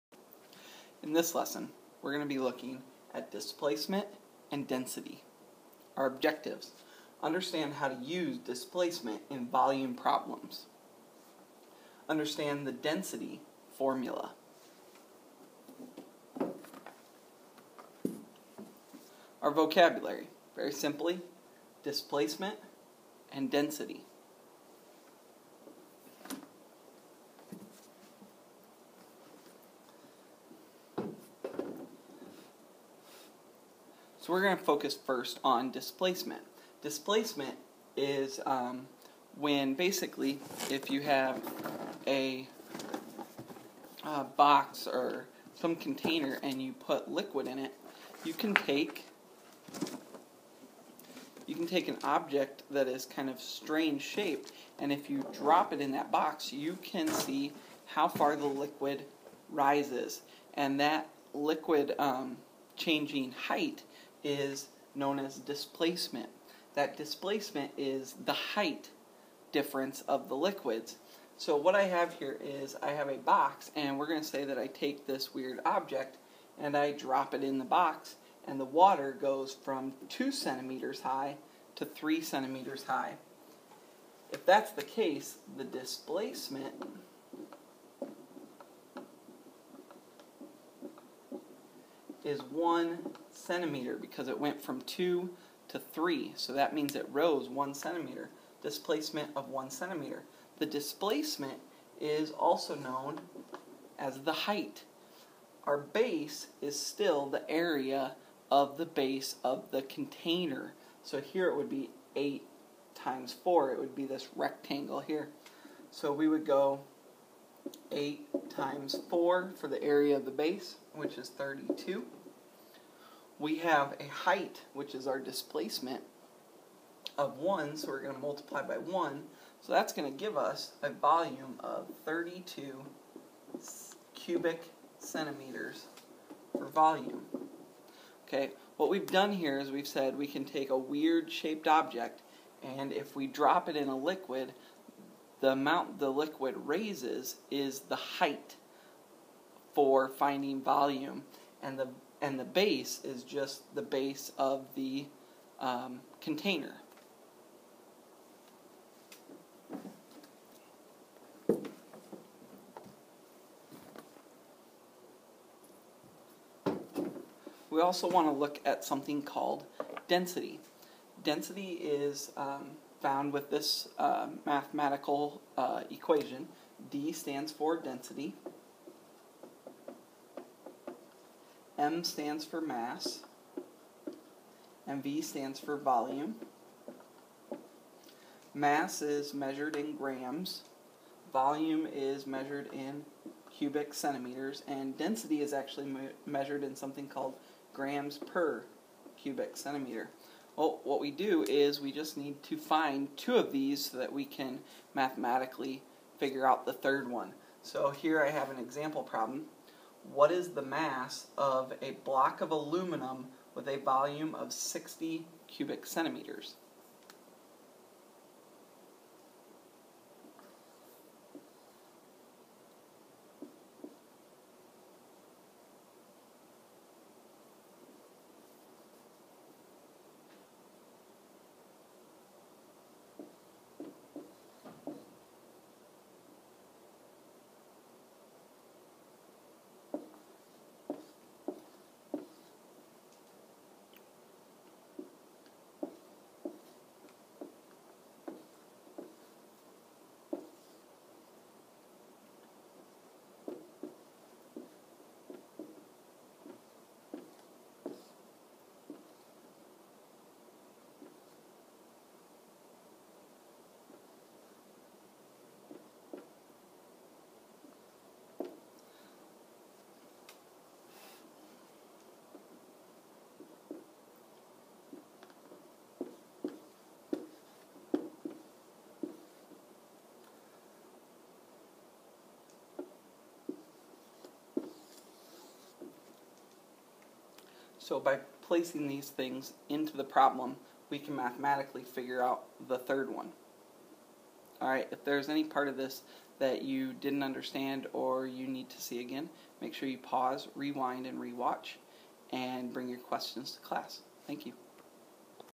Unit H Video 4 - Displacement & Density
How to use displacement within volume formulas. How to use the density formula.

Objectives -
1.  Understand how to use displacement in volume problems.
2.  Understand the density formula.

Vocabulary -
1.  Displacement
2.  Density